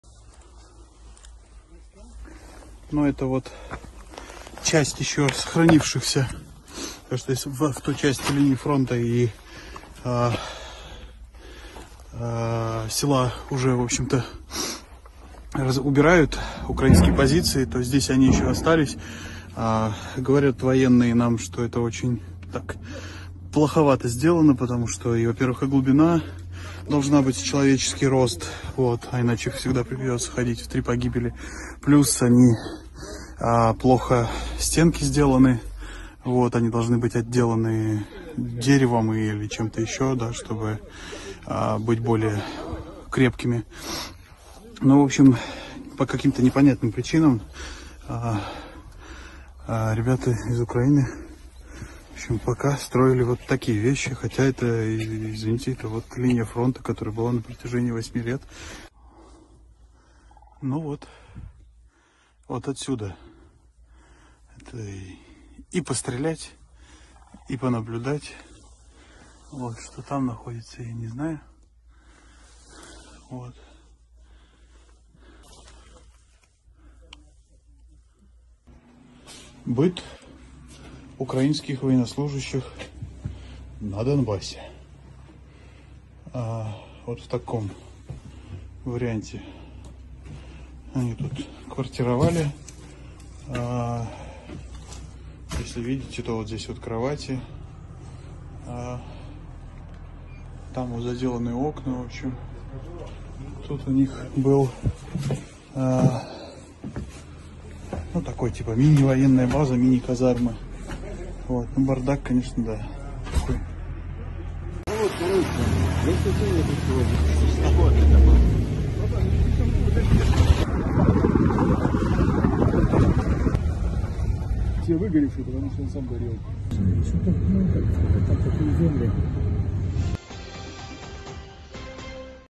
ДневникМаслака #6 Ситуации в Донецке
ДневникМаслака Day 6: Это бывшие позиции ВСУ на линии фронта, которая была 8 лет. Ещё недавно в селе Гранитное стояли подразделения националистического батальона Правый сектор(организация запрещена в России) и ВСУ. Окопы расположены у реки, которая была непосредственно линией фронта. В окопах сохранился блиндаж, траншеи, а рядом в бывшем жилом 1-этажном доме была казарма и склад боеприпасов.